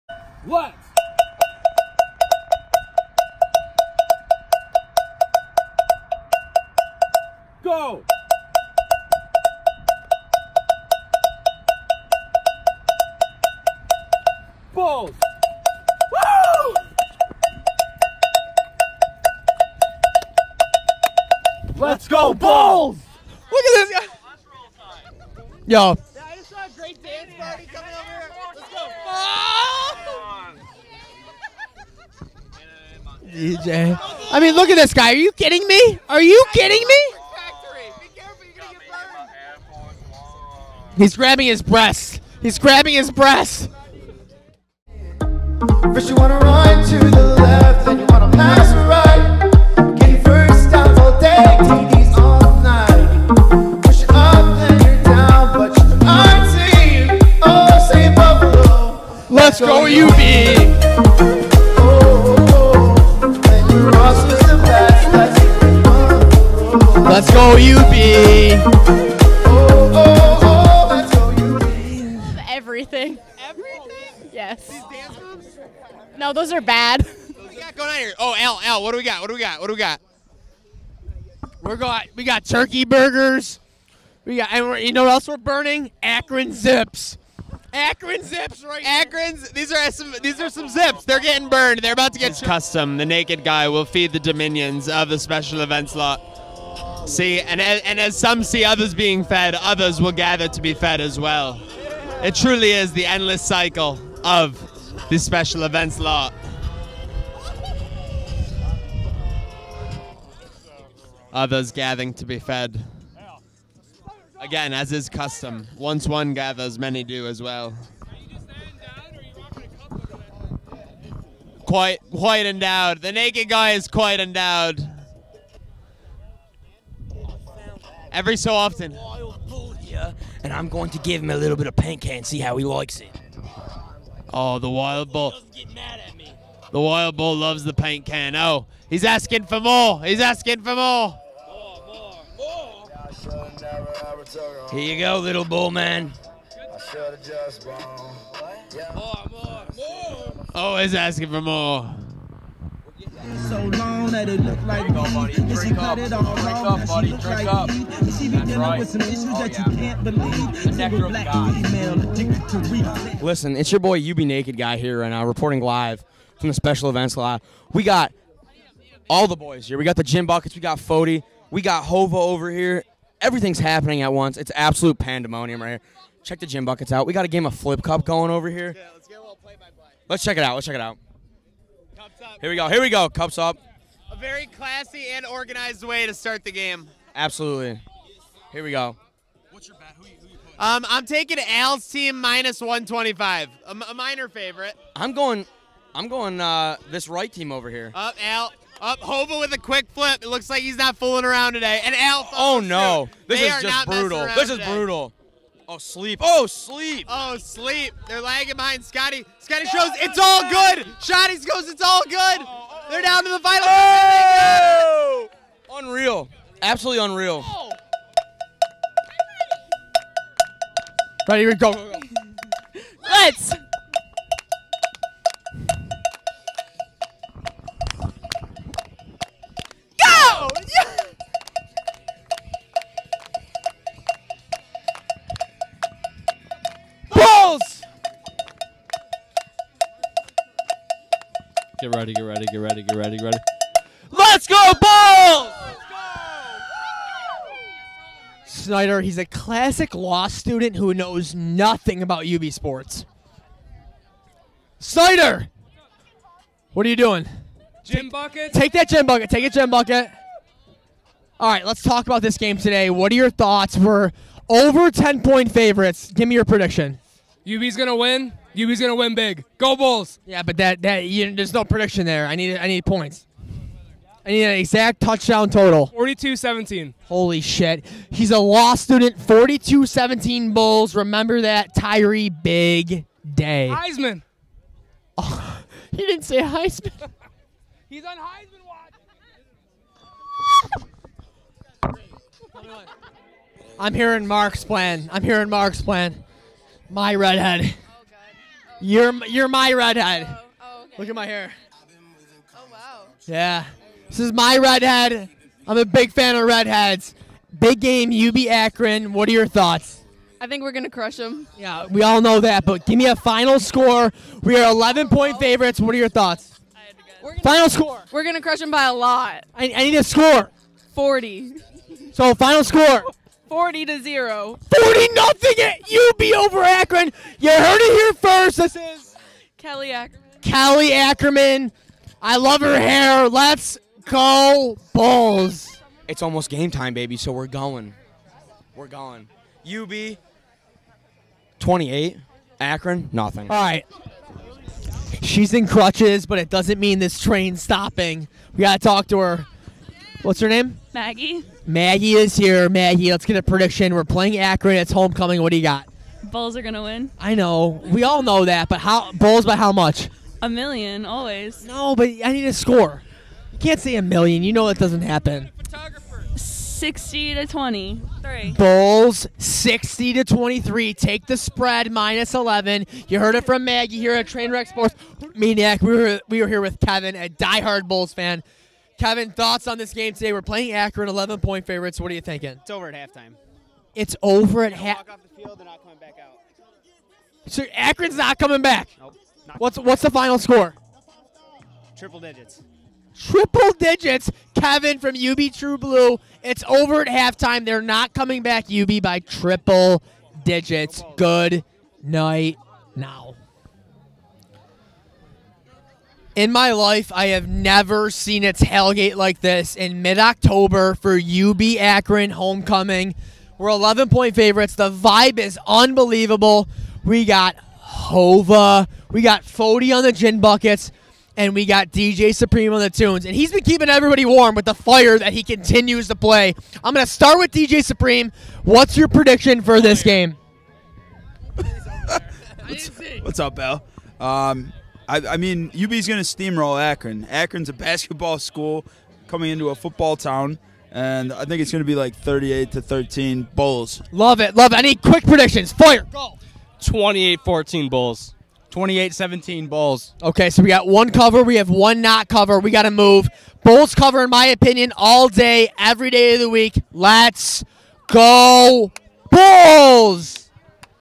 UB Akron Tailgate Report
After a convincing win at Central Michigan, the Bulls hosted the Akron Zips this past Saturday in search of bowl eligibility and their best start in nearly 60 years! The Wrecking Crew invaded the Special Events Lot for insight from the True Blue on how the Bulls would match up against the Zips.